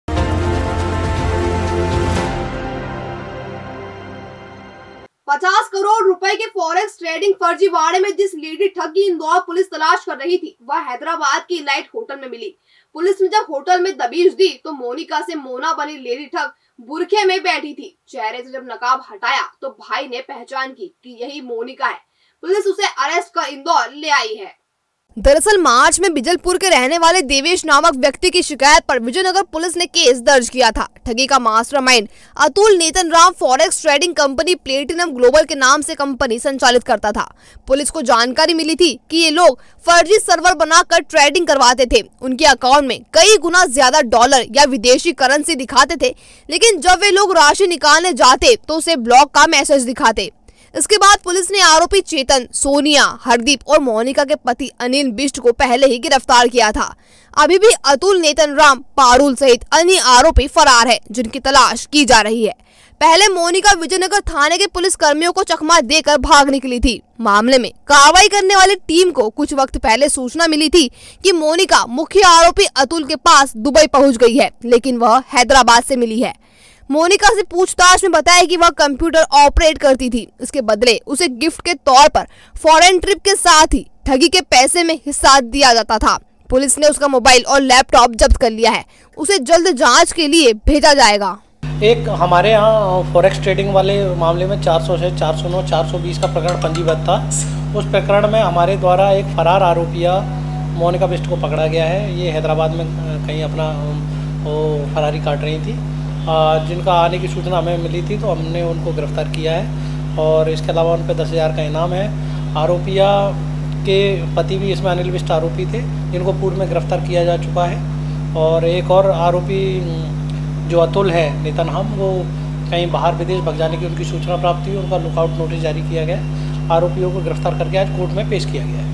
Indore news: Mona बनकर छिपी Lady ठग Monika Under arrest | Abhay News
50 करोड़ रुपए के फोरेक्स ट्रेडिंग फर्जीवाड़े में जिस लेडी ठग की इंदौर पुलिस तलाश कर रही थी वह हैदराबाद के इलाइट होटल में मिली। पुलिस ने जब होटल में दबिश दी तो मोनिका से मोना बनी लेडी ठग बुर्के में बैठी थी।

About Channel